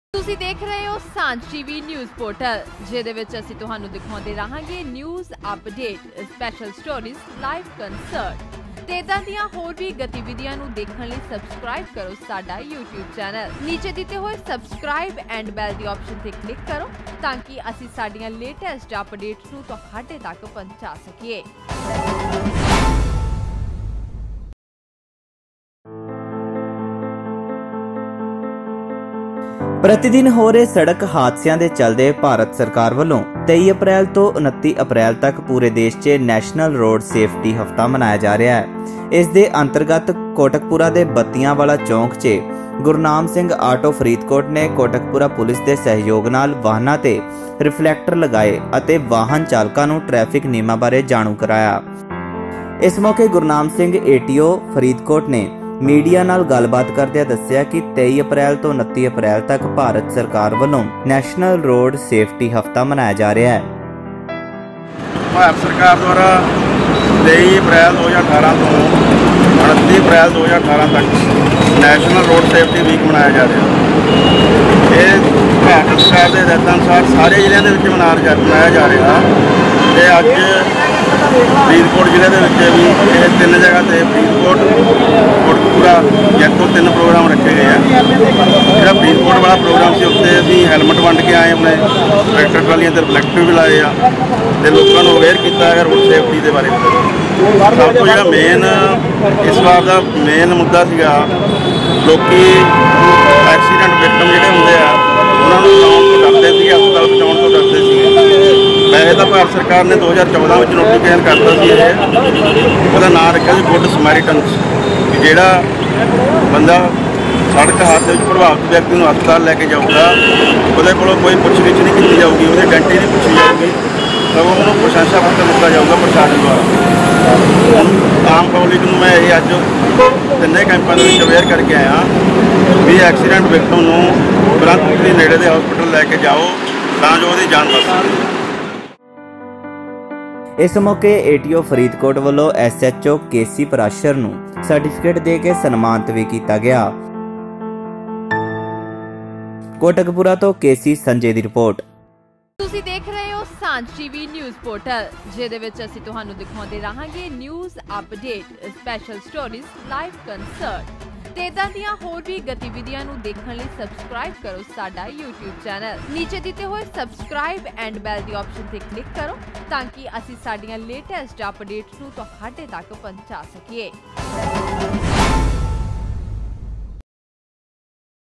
ਕੋਟਕਪੂਰਾ 'ਚ ਪੁਲਿਸ ਵਲੋਂ ਵਾਹਨ ਚਾਲਕਾਂ ਨੂੰ ਟ੍ਰੈਫਿਕ ਨਿਯਮਾਂ ਸਬੰਧੀ ਦਿੱਤੀ ਜਾਣਕਾਰੀ
Marriage Ceremony!!!